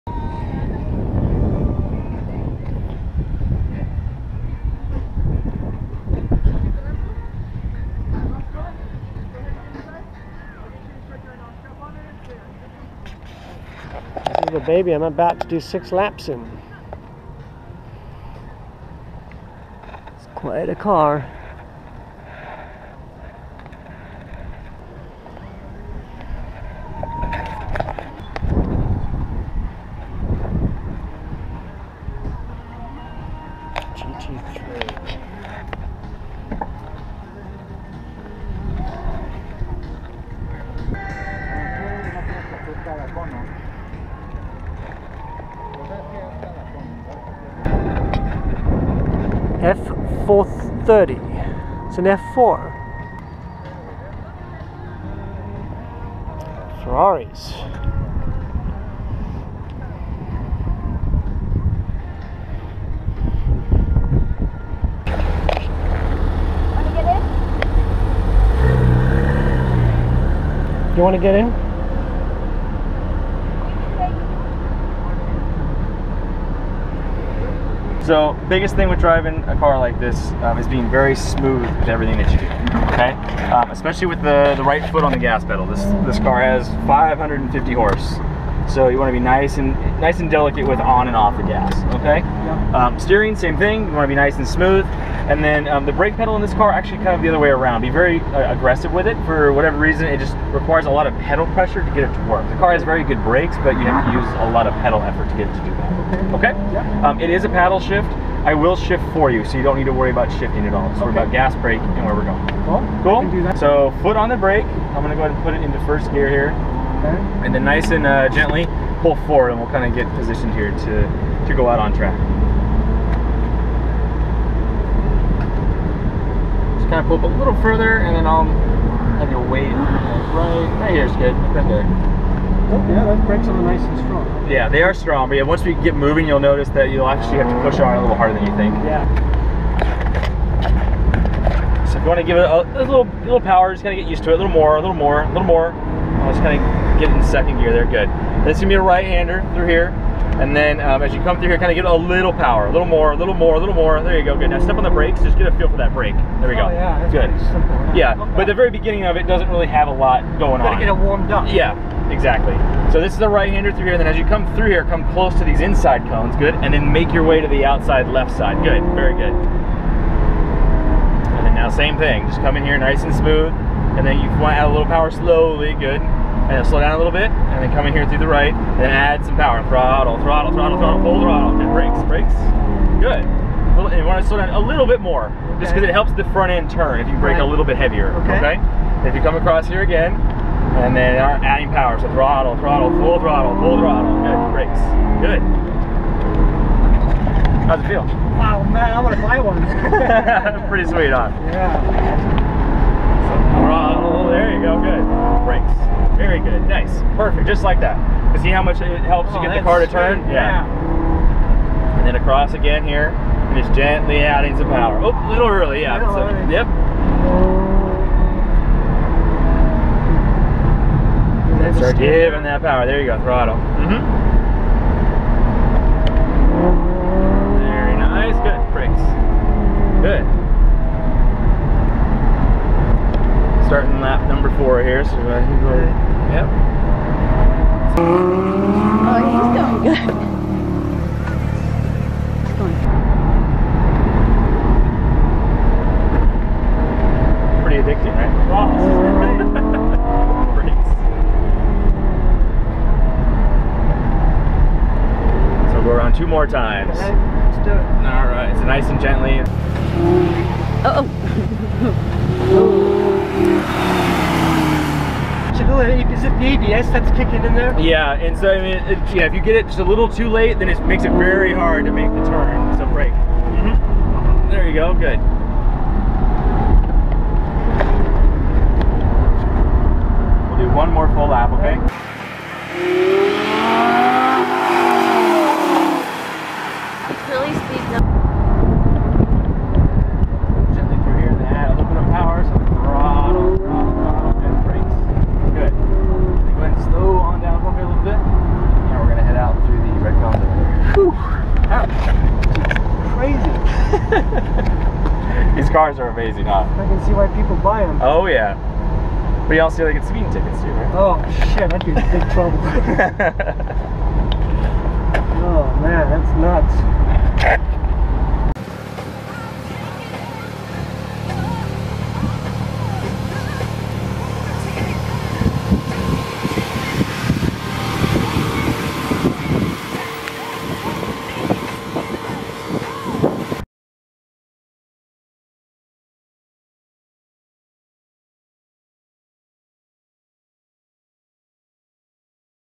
Lamborghini Gallardo 6 Laps
Got this certificate for driving this exotic sports car.  Car was amazing, just the track (parking lot) was too small at the Sleep Train Ampitheater and could not get the car out of 2nd gear out of 6 gears.

The experience was great and would recommended it to anyone.  Check out their website.